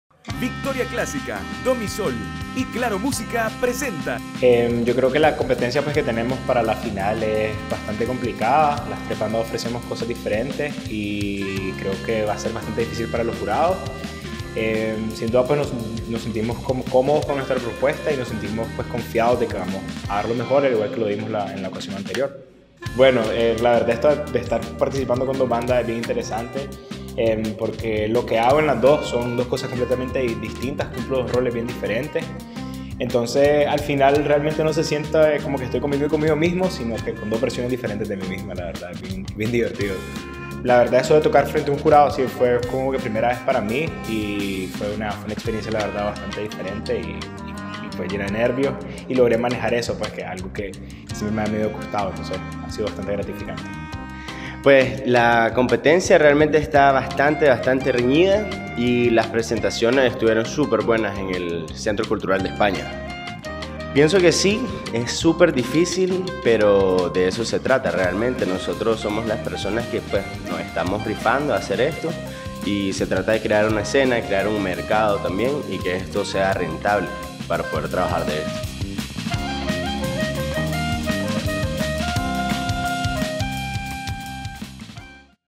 Artificios - finalistas concurso Emergente
¿Estás list@ para conocer más de Artificios y de su participación en ConcursoEMERGENTE 🔥😎?
¡Estate atent@ a nuestras redes, participá en las dinámicas y ganá premios!
No olvidés que éste próximo 23 de Octubre van a estar en el escenario demostrando toda la preparación que han recibido en Proyecto Emerge desde los espacios formativos y sesiones de coaching a cargo de Centro de Estudios Musicales
¿Vos apoyás la escena musical emergente de Nicaragua?